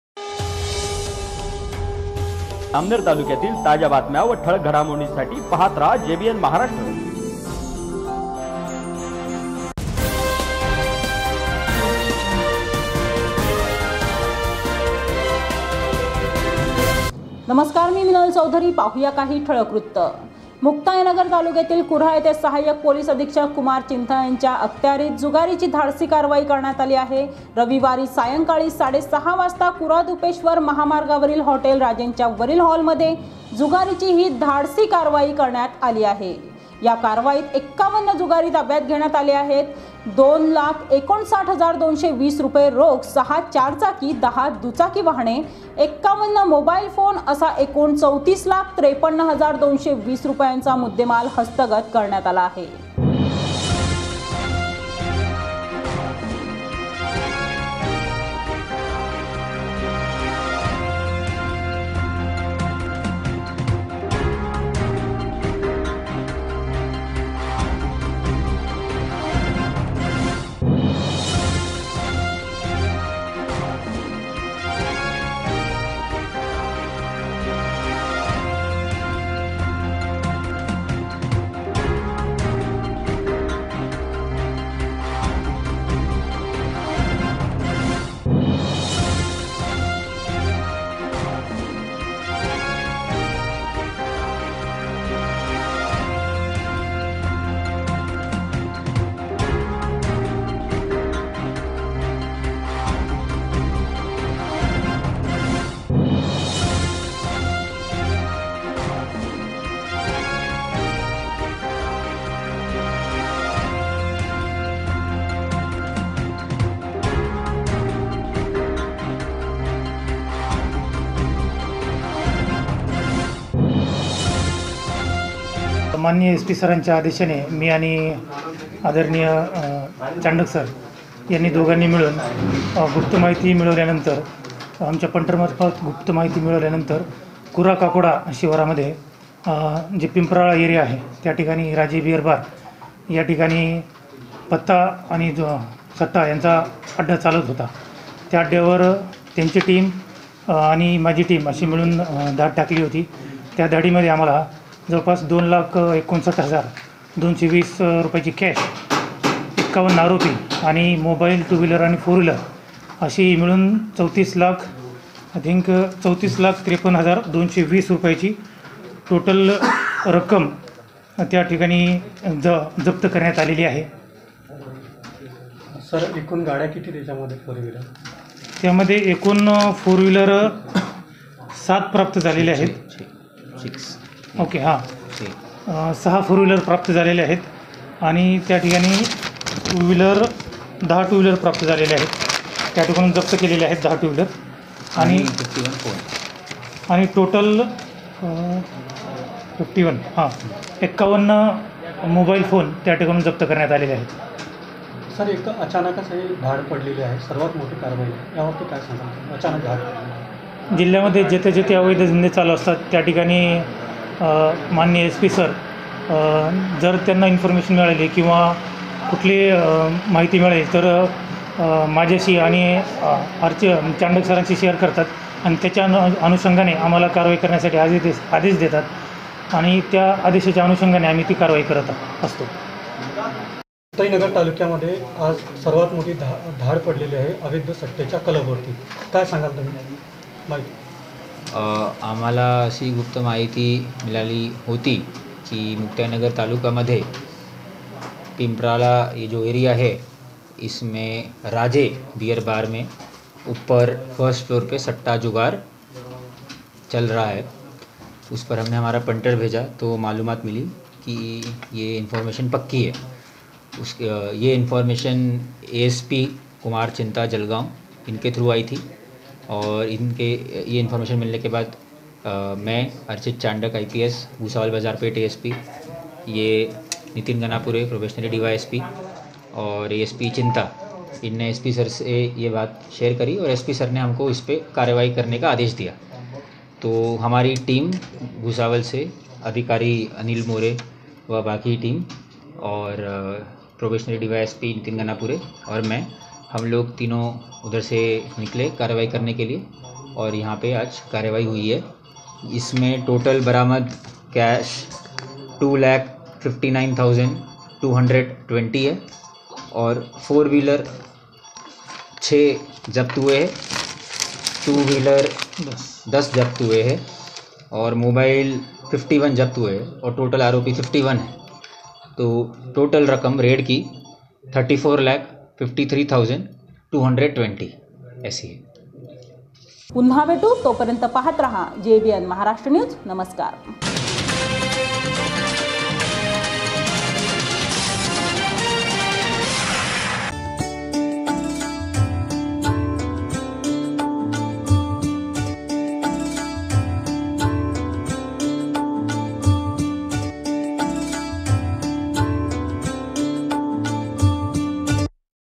jbn maharashtra news jalgaon
पोलिसांची हॉटेल वर धाड,51 जुगारी ताब्यात.तर 2 लाख 59 हजार 220 रुपये रोख, 6 चार चाकी, दुचाकी दहा वाहने, 51 मोबाईल फोन असा एकूण 34 लाख 53 हजार 220 रुपयांचा मुद्देमाल हस्तगत करण्यात आला आहे